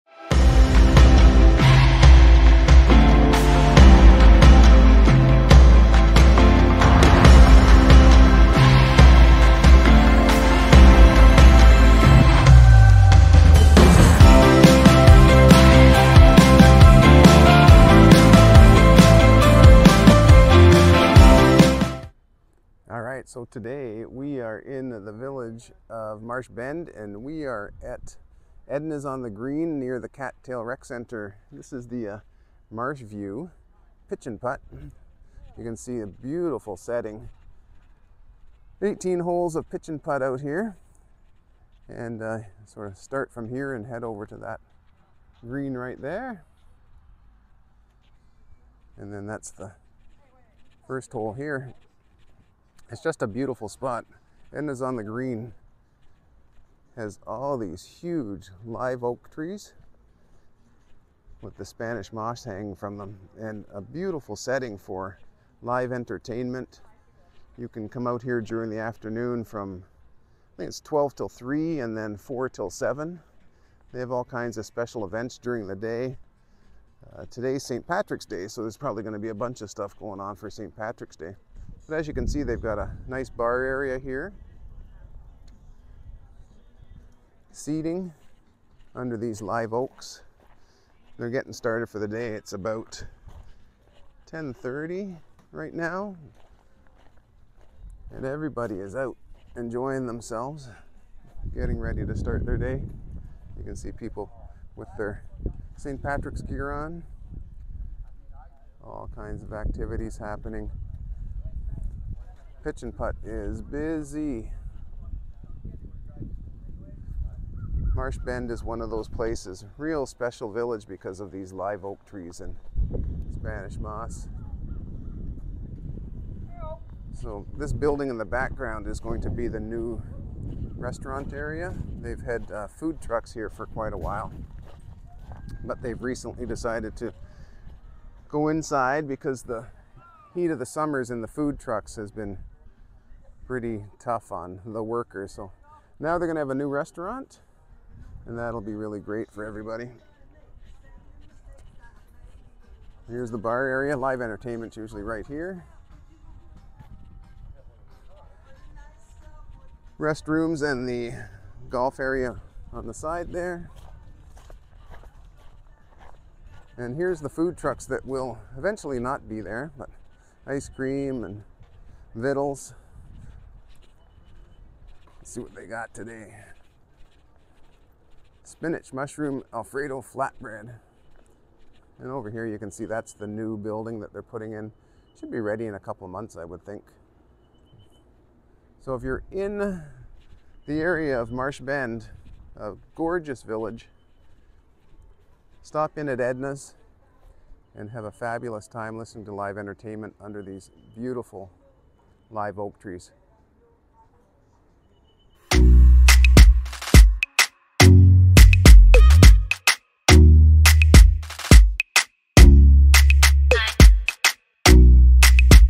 The Villages Florida, Ednas on the Green and Marshview Pitch and Putt
Lets have a look at Ednas on the Green and Marshview Pitch and Putt in the Villages Florida. This beautiful setting under the Live Oaks is the perfect spot to spend an afternoon listening to Live music, or playing a round of Pitch and Putt